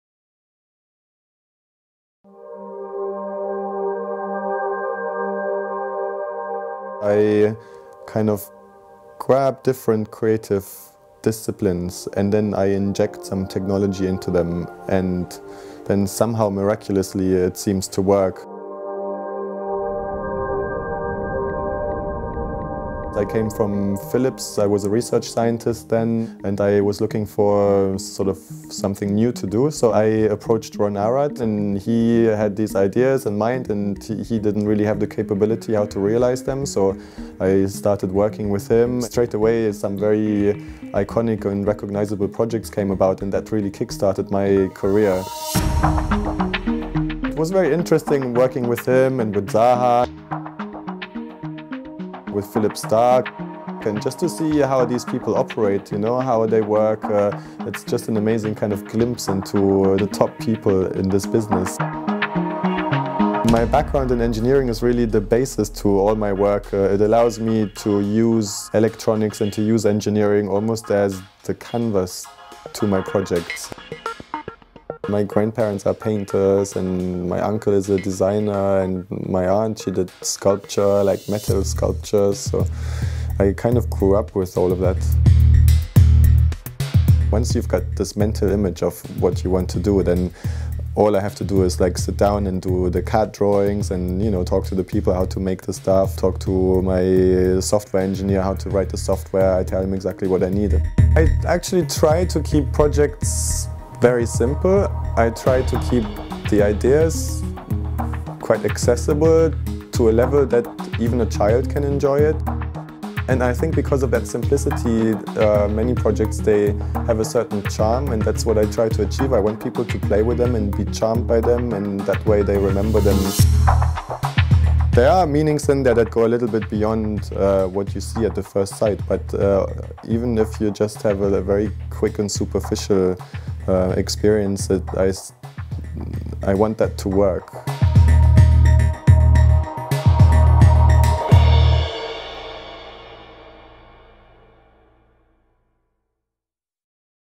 Moritz Waldemeyer's Designs
Moritz Waldemeyer, a German designer first worked as a research scientist at Philips before moving into design after approaching architect Ron Arad to work on a collaborative project. Waldemeyer fuses electronics and engineering with art and design, making way for fascinating collaborations with the world's leading architects like Zaha Hadid and Ron Arad, and artists like U2 and OK GO.